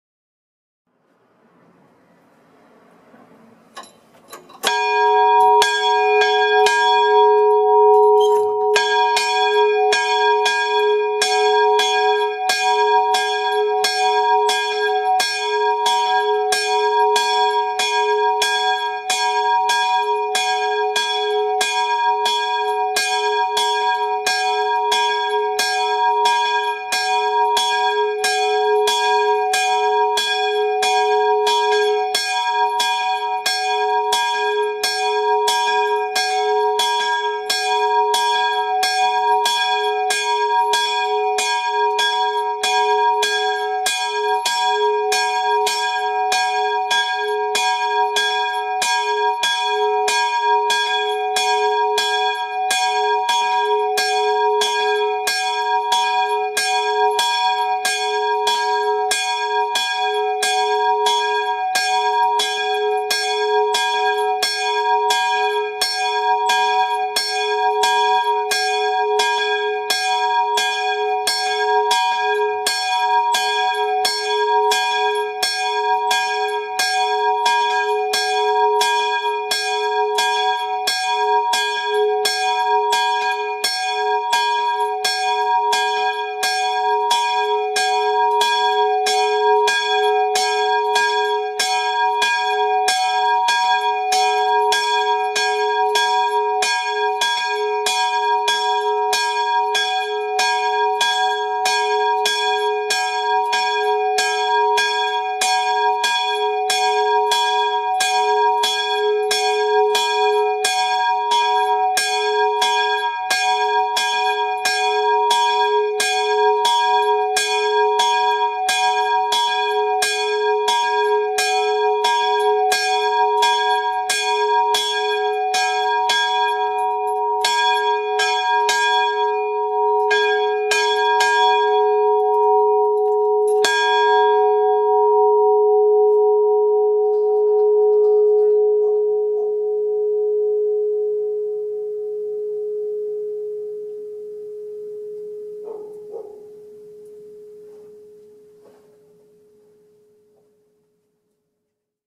Jaworzno (Góra Piasku) - Dzwon kaplicy pw. św. Stanisława
Kaplica znajduje się na terenie parafii Św. Elżbiety Węgierskiej w Jaworznie - Szczakowej
Dzwon
Imię: Maryja (prawdopodobnie)
Ton: gis"
Waga: ~ 80 kg
Średnica: 50 cm
Wysokość (z koroną): 43 cm
Wysokość korony: 5 cm
Wykonany w: K. Schwabe, odlewnia dzwonów w Białej
Rok odlania: 1928